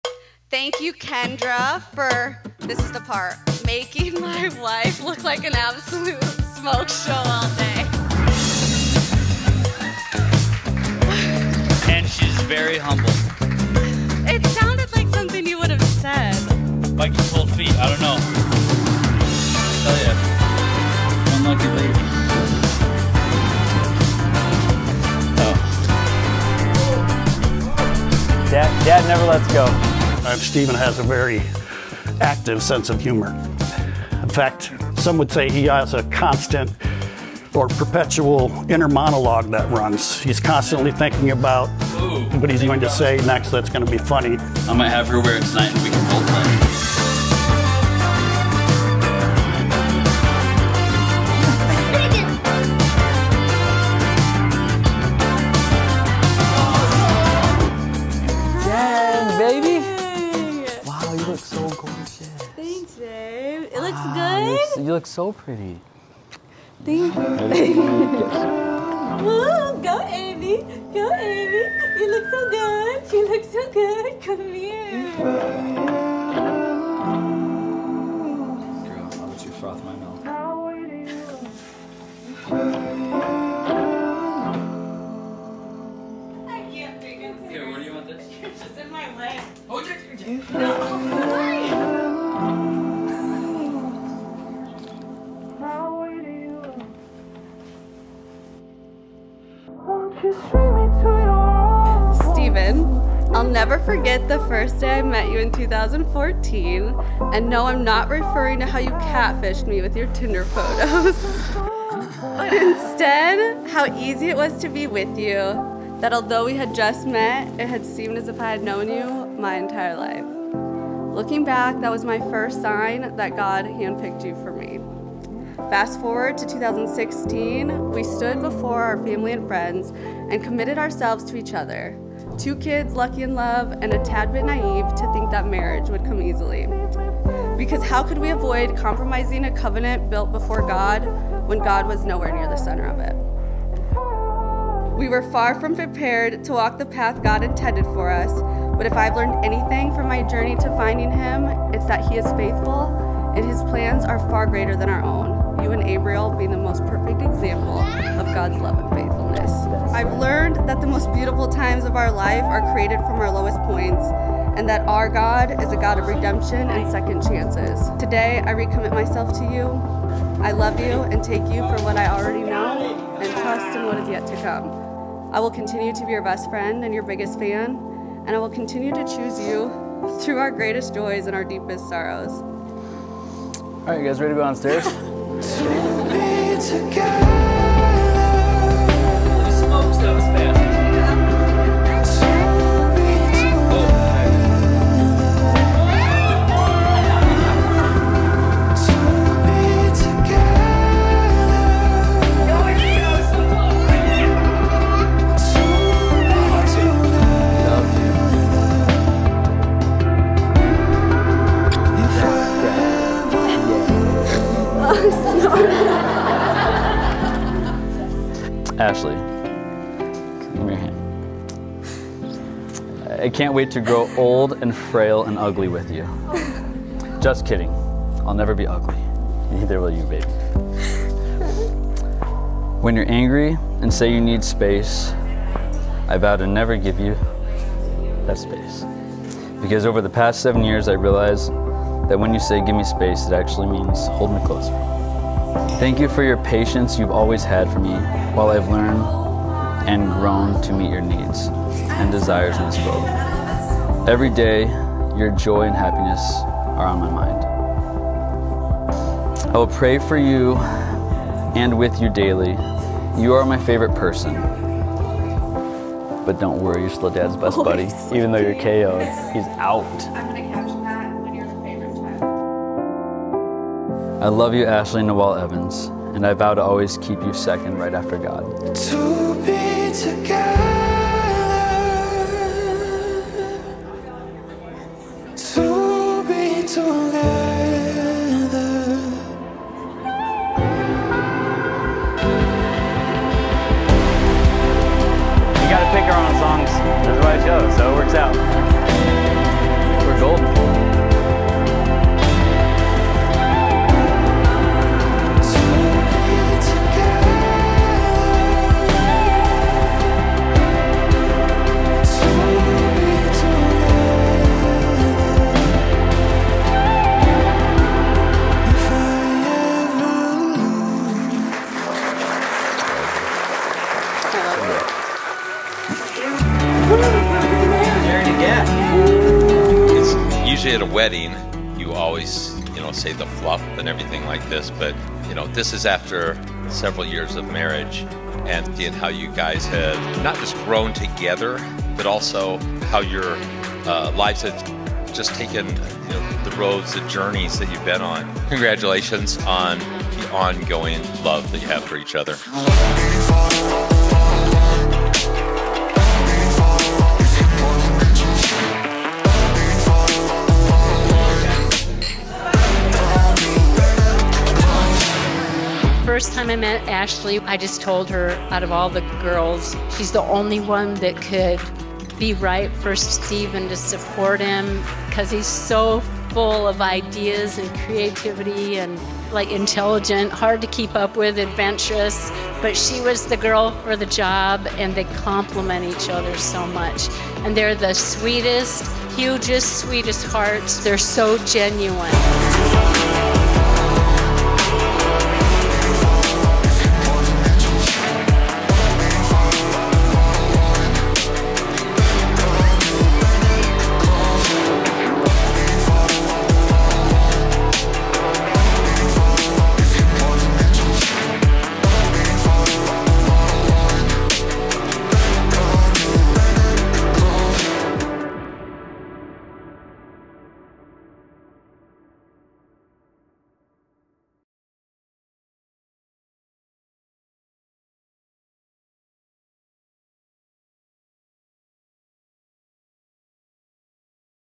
VOW RENEWAL | Highlighting our special day
We had a chance to say "I Do" for a second time and it couldn't have been more magical! Enjoy our Vow Renewal highlight video and let us know if you want a FULL Vlog style video!

Videography: @travisandjonifilms

Venue: @carriagehousecr
Design and planning @anneandellis
decor: @1111eventco
Photographer: @forevergreenstudios
DJ: @djnathankabat
Bar: @dave_themixologist with The Carriage House
Hair and Makeup: @kendraarhus
Floral: @ellabankblossoms
Catering: @peppercornfoodco
Jewelry: @kayjewelers
Balloon Backdrop: @poppinpartiesco
Cheesecake: @sweetcheesecake1216
Cake pops: @magnoliabakery
Signage: @allegraiowacity
Spray tans: @bare_tones
Dress: @hopesbridal